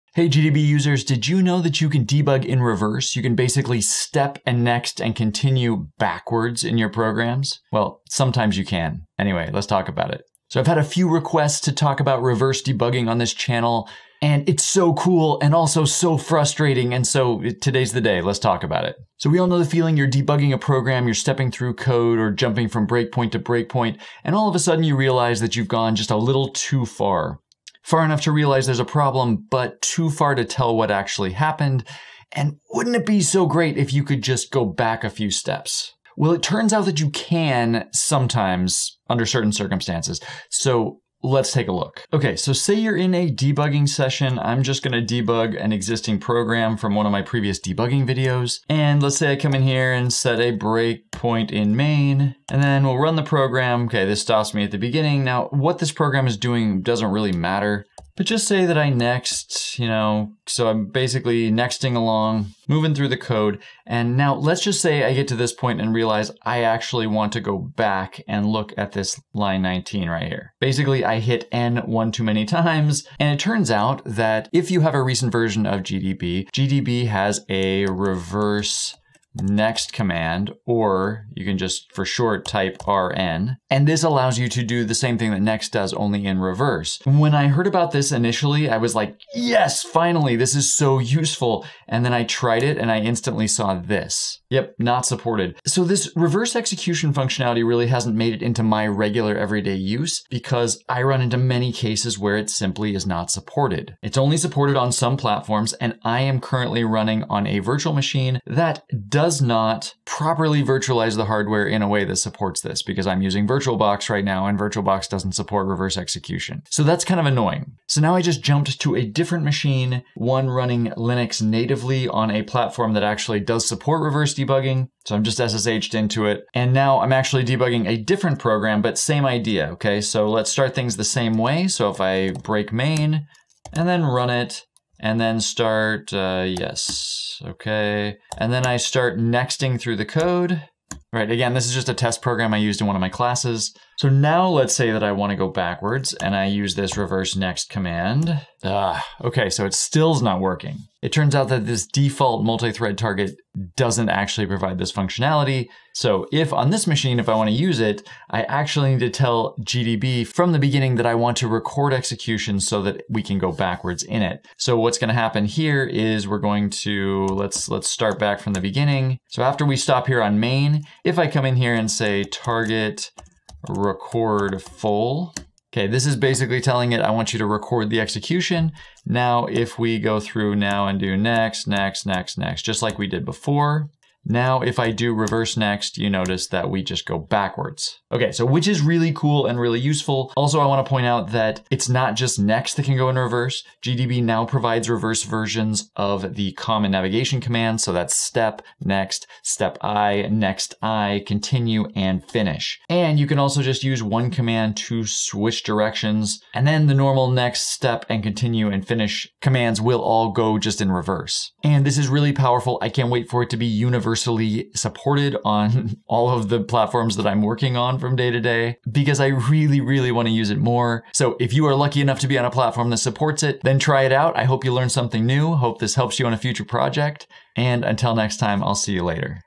How to reverse execution when debugging in GDB.
How to reverse execution when debugging in GDB // When debugging we often have to restart our process because we accidentally stepped or nexted too far. Now, if you're on a platform that supports it, you can reverse execution and step back a few steps. This video shows you how.